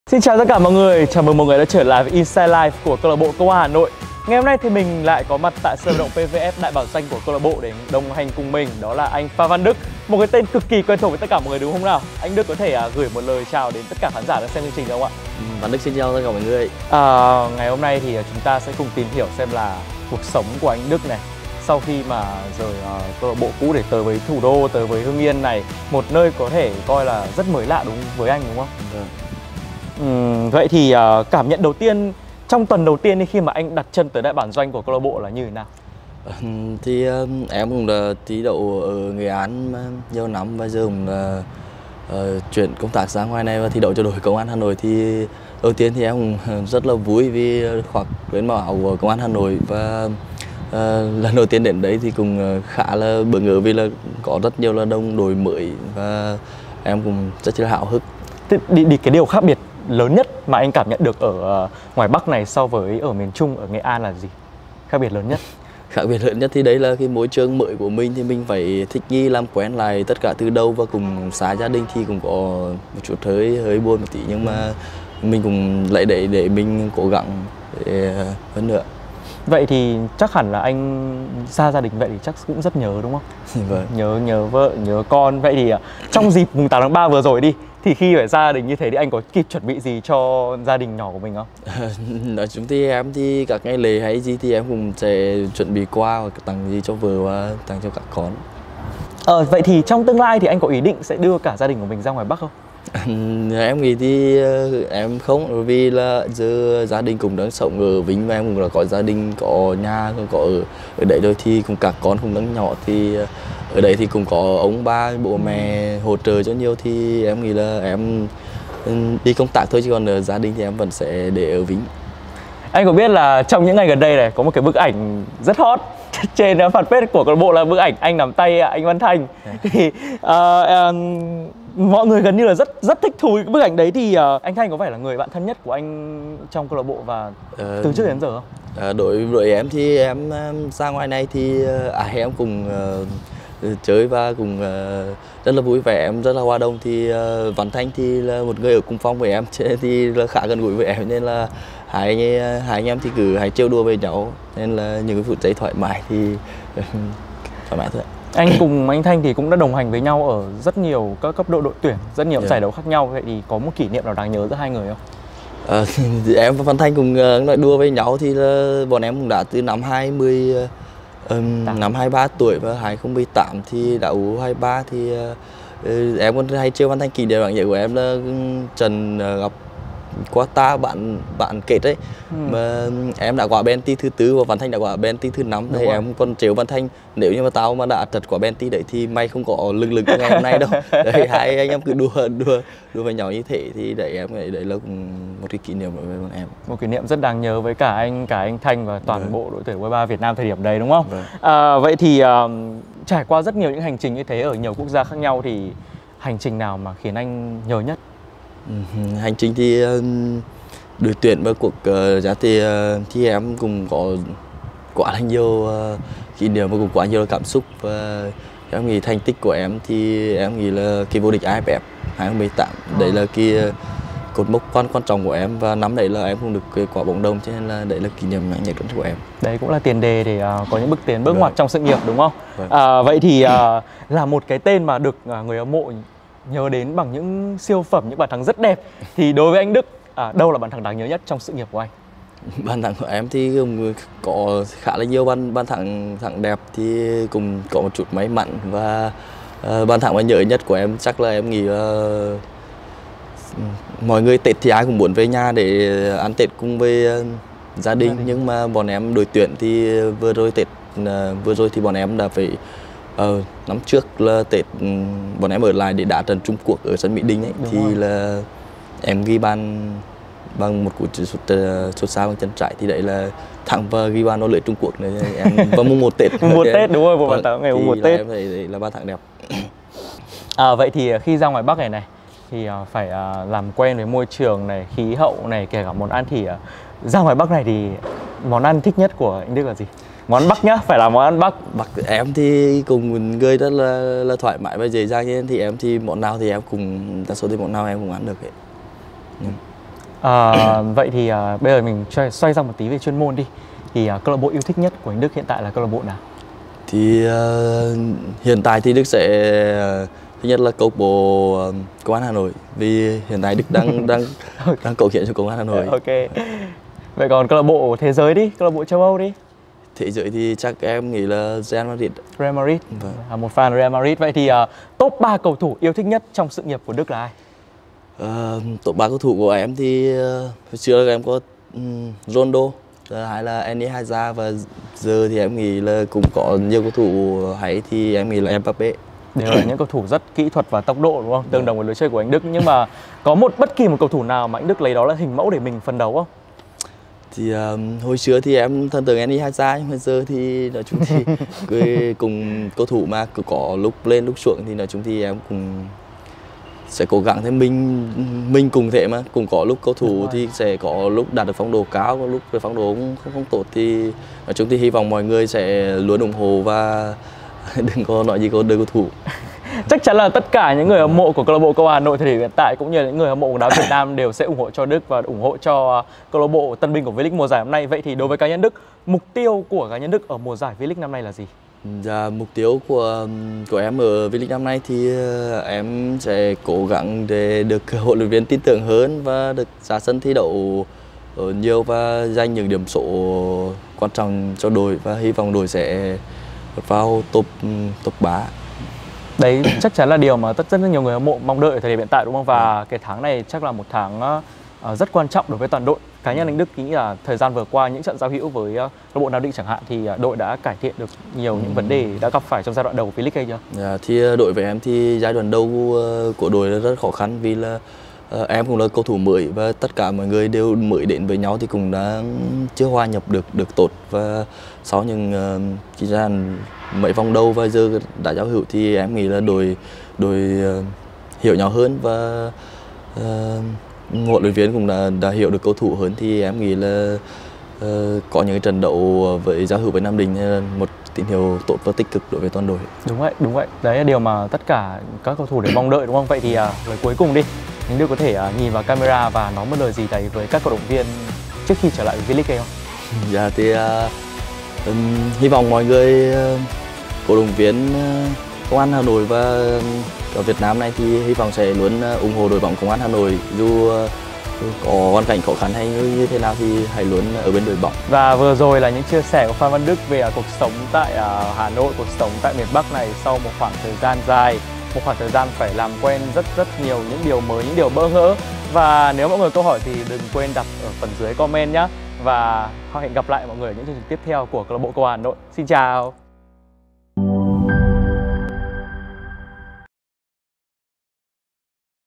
ĐỒNG ĐỘI THÂN THIẾT NHẤT VỚI PHAN VĂN ĐỨC Ở CLB LÀ... 👀 | CAHN FC - INSIDE LIFE
Phan Văn Đức đã có những chia sẻ về dự định cá nhân và gia đình, về mối quan hệ với những người đồng đội cũng như mục tiêu sắp tới tại CLB Công an Hà Nội.

© Bản quyền thuộc về CLB Công An Hà Nội